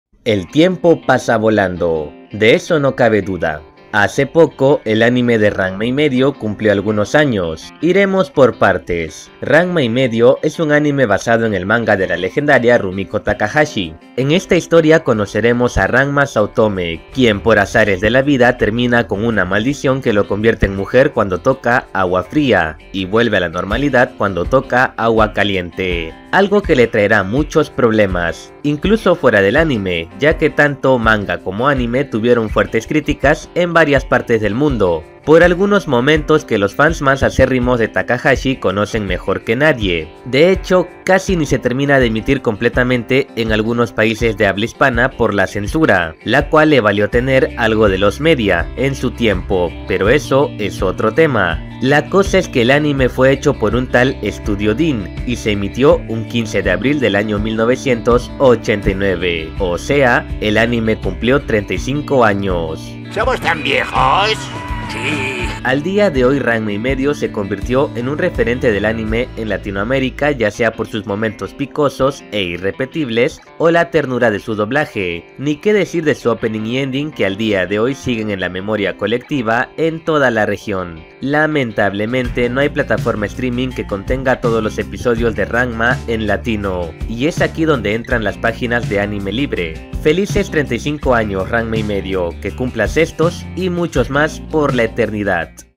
THE SAM NEWS #80: El anime de Ranma 1/2 está de aniversario
El anime de Ranma 1/2 cumplió 35 años hace poco y en este vídeo hablaremos un poco sobre una de las obras cumbre de la insuperable Rumiko Takahashi.